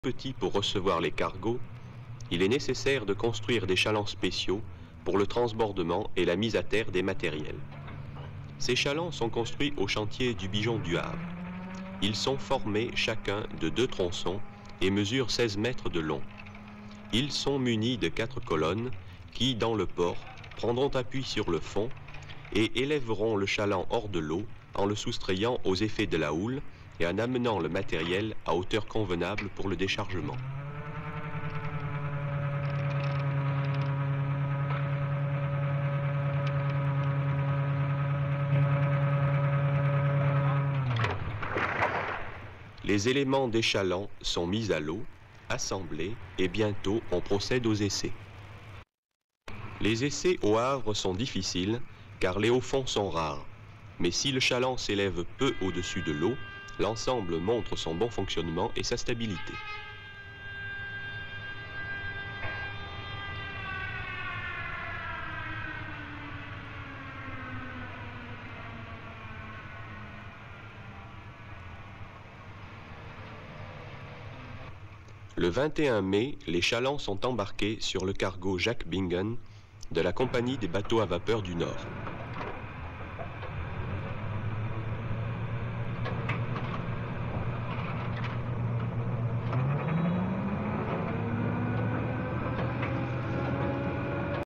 L'embarquement, après la construction au Havre, des chalands pour l'île de Florès le 5 décembre 1966
Afin d'observer la trajectoire des fusées à longue portée tirées des Landes en France, il est décidé d'implanter une base sur l'île de Florès dans l'archipel des Açores. Le port de cette île n'étant pas adapté pour l'accueil des cargos et le déchargement du matériel lourd nécessaire à la construction de cet "observatoire", la construction des chalands est confiée à un chantier Havrais.